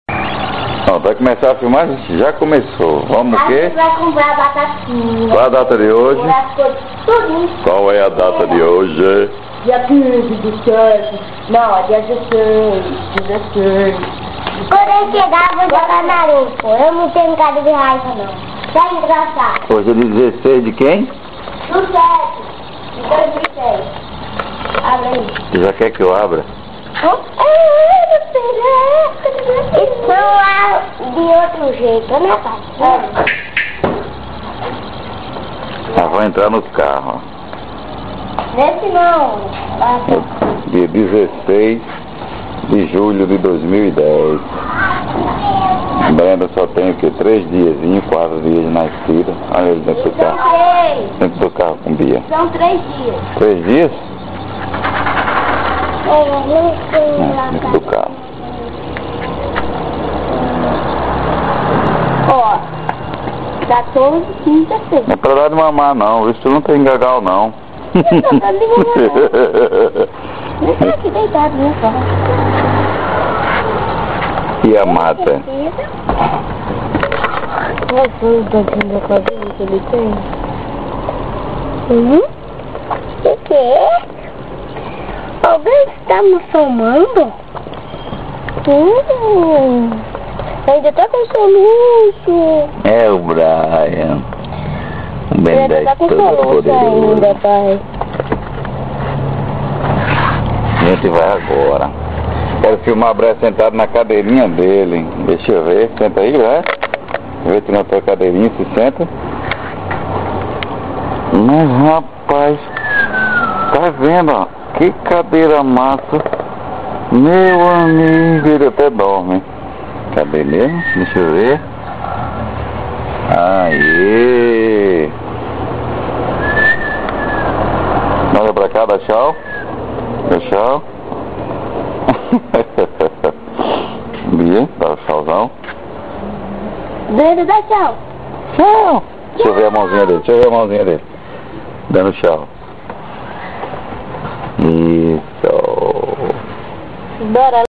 Beatriz e Brian Thompson apresentam Brendan
Meninos apresentam o irmão recem nascido.